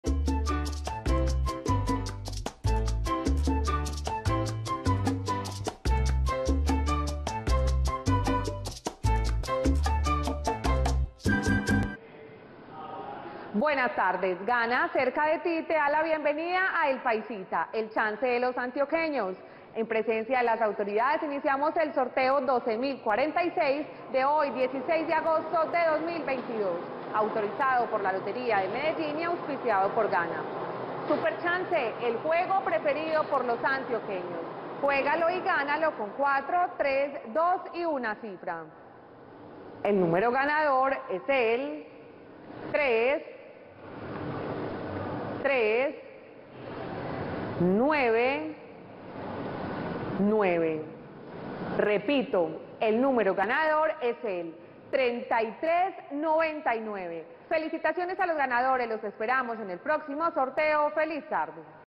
PAISITA DÍA Resultados del MARTES 16 DE AGOSTO de 2022 ✅🥇🔥💰
Hola buenas tardes, te presentamos los resultados de las loterías y chances en Colombia, vamos con el Resultados del Chance Paisita día del MARTES 16 DE AGOSTO de 2022.

Resultado Lotería de Cundinamarca MARTES 16 DE AGOSTO de 2022
Resultado Lotería del Tolima MARTES 16 DE AGOSTO de 2022